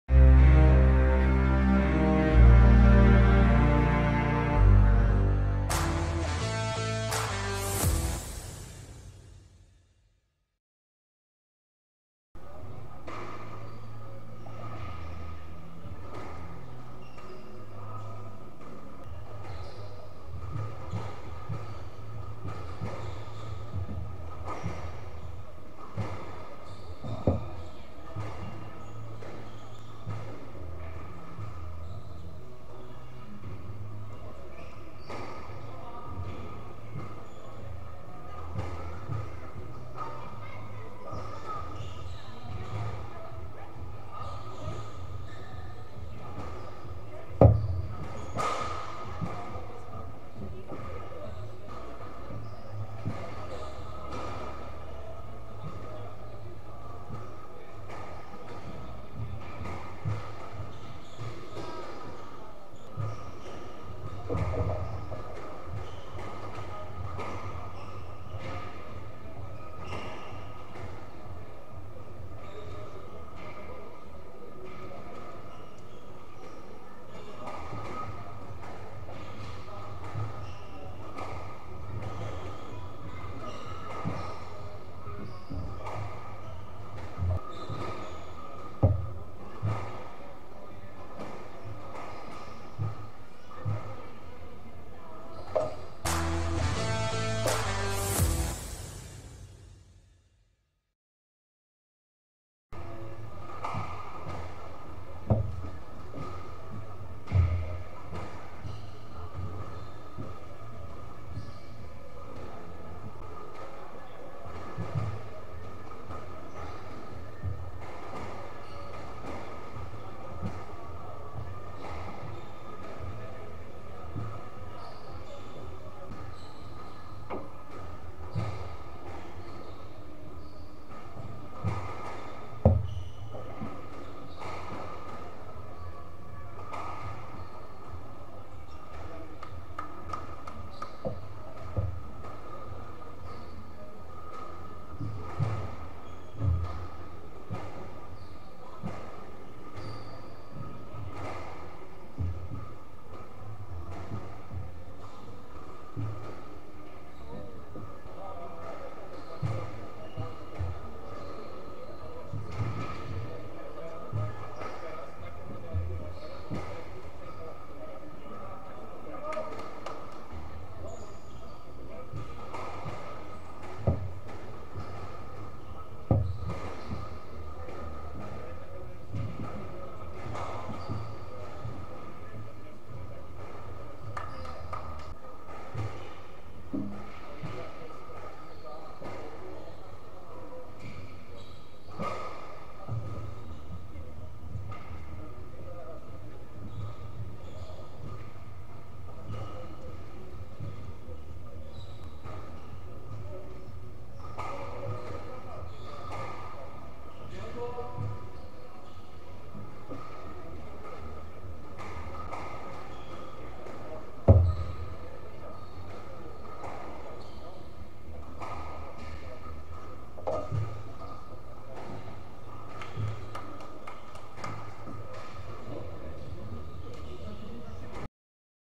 Squash - URODZINY+FINAŁ IGRZYSK SQUASHA - Patryk vs Rafał (skróty)
A jeśli ci się spodobało i chcesz abym tworzył więcej takich treści, oraz żeby były lepsze, możesz mnie wesprzeć tutaj